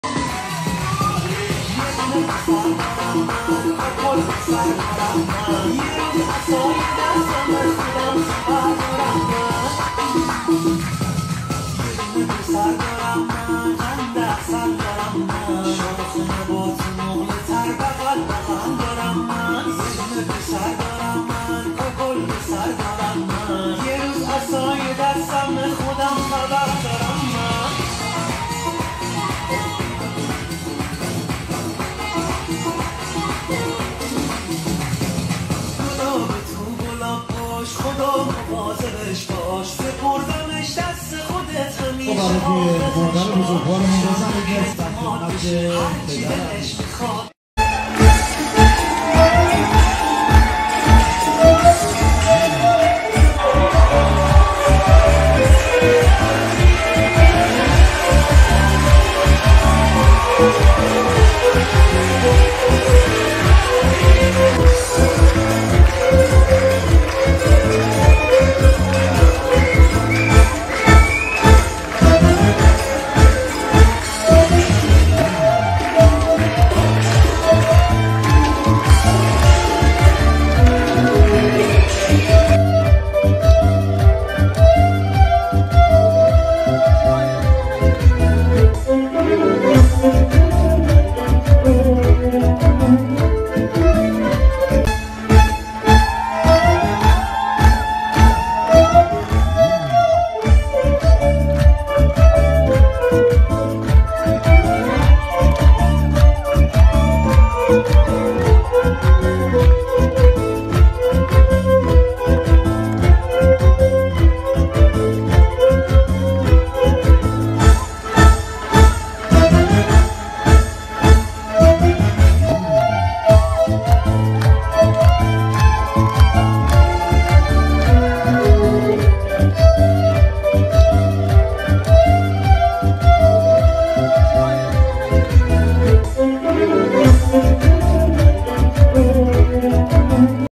رقص جذاب اقا داماد با مادرشون،طفلک عروس خانم که نیستن،چی بگم
با ارزوی خوشبختی برای همه عروس ودامادهای عشقمون،خوشبخت بشین